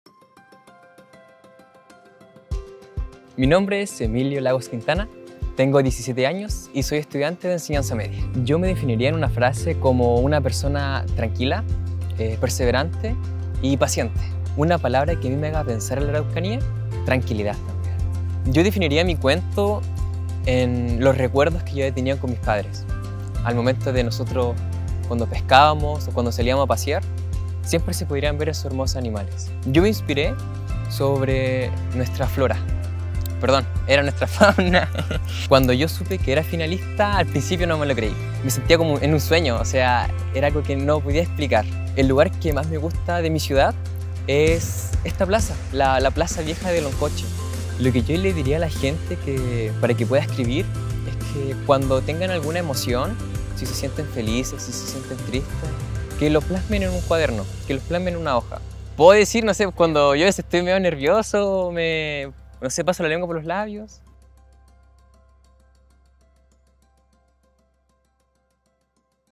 Premio al Talento Joven | Araucanía en 100 Palabras V
Con su cuento DESDE LOS RÍOS HASTA LOS CIELOS, el joven de 17 años oriundo de Loncoche, Emilio Lagos, ganó el Premio al Talento Joven de Araucanía en 100 Palabras.